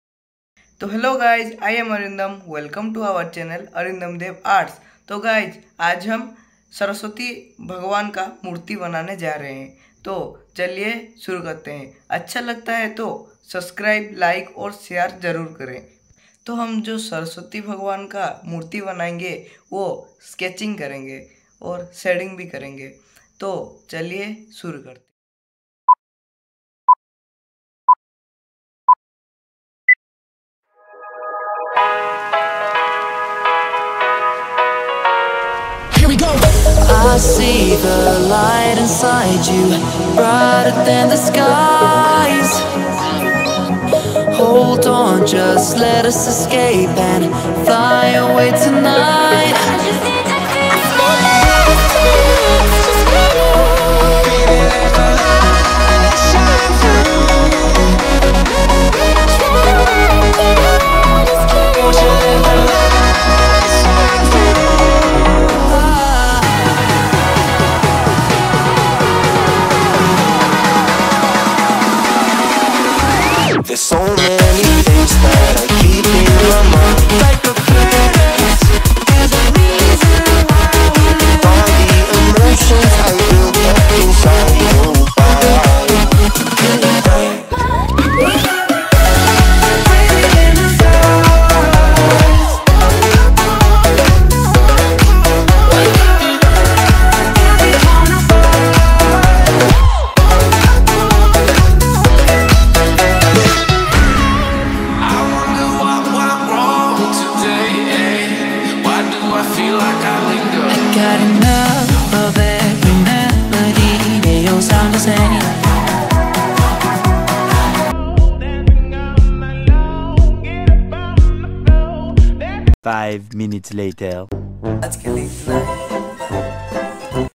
How to draw Saraswati Devi ||Saraswati maa drawing || Arindamdev Arts
Hello guys
Today I uploaded the video how to draw saraswati maa drawing.